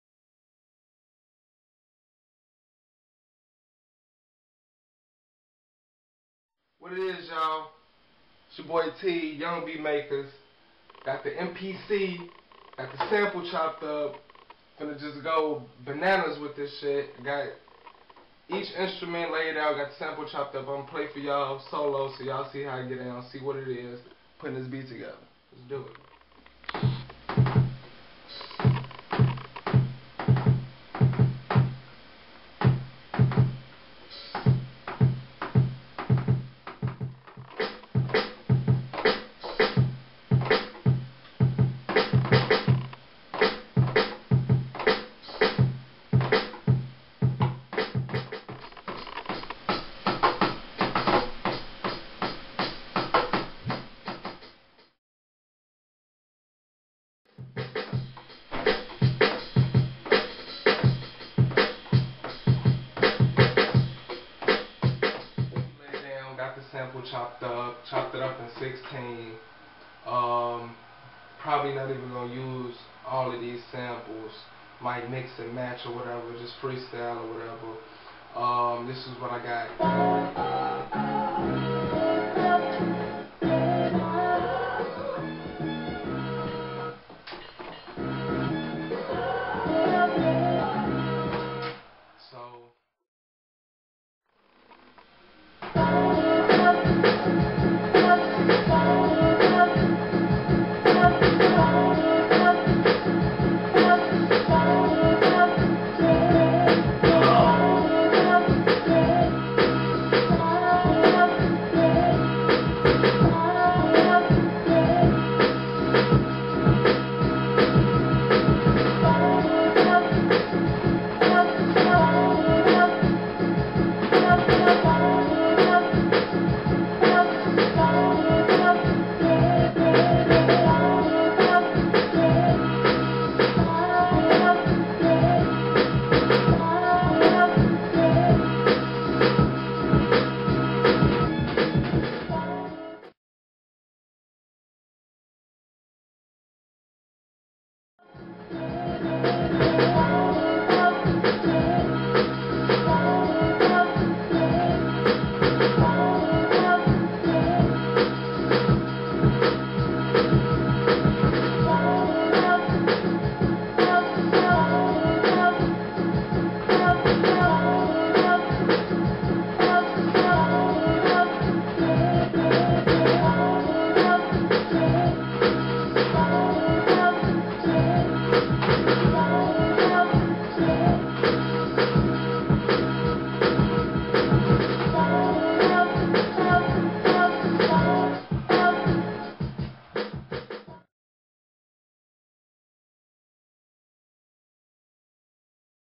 TheYBMTV: Making A Beat With MPC2000
808 Making A Beat on The Mpc2000XL, The Sample is Chicago Own "Chi-Lites" Comment and Rate Thanks! Follow Me @Tha808kid